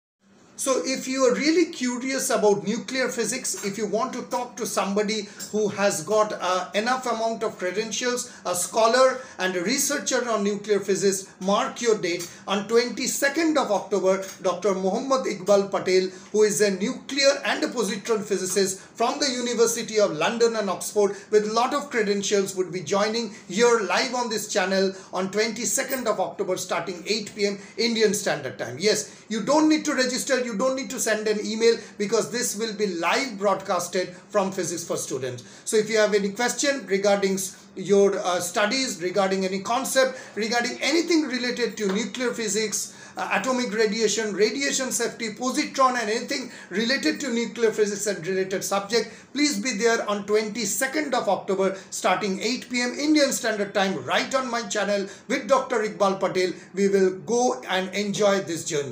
Live session on Nuclear Physics | Nuclear physics lecture
Do you have questions in your mind for Nuclear physics. Here is a great opportunity for you. 22nd. October '2023, Dr. Mhd.Iqbal Patel will be coming Live on this channel from London to answer all your questions. He is a Nuclear physicist from London University and has taught at University of Oxford. Be there on 22nd.October starting 8:00 pm IST, here on Physics for Students.